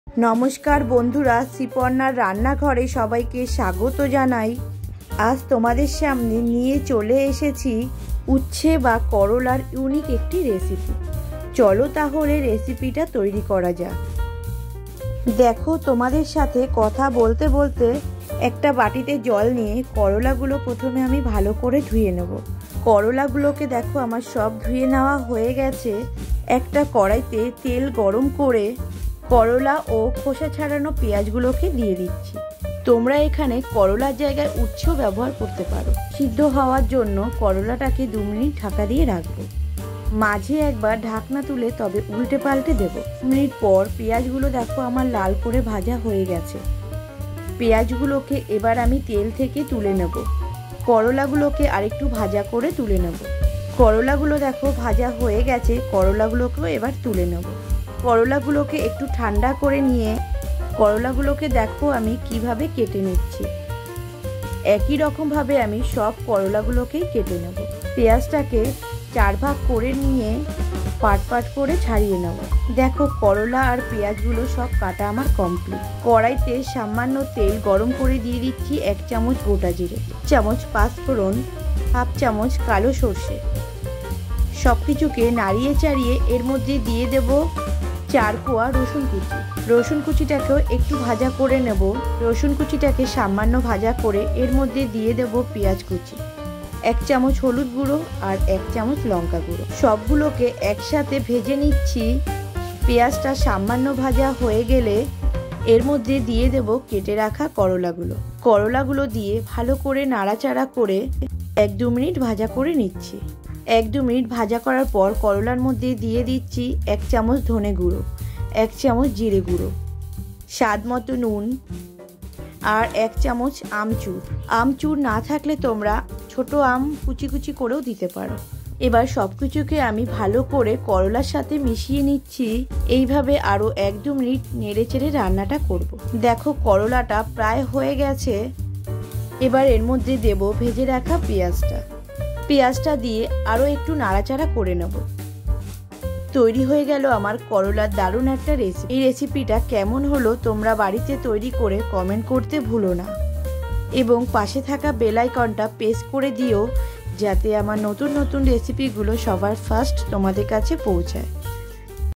করোলা এই পদ্ধতিতে রান্না করলে বাচ্চারাও চেয়ে খাবে/Korolar recipe bengali sty@sriparnarrannavlog3674
করোলা এই পদ্ধতিতে রান্না  করলে বাচ্চারাও চেয়ে খাবে/Korolar recipe bengali sty‎le@Sriparnar Rannaghar

Korola
onion
Girlic
jeera powder
Dhaniya powder
Amchur
chilli powder
Haldi powder
Oil
panchfrong
mustered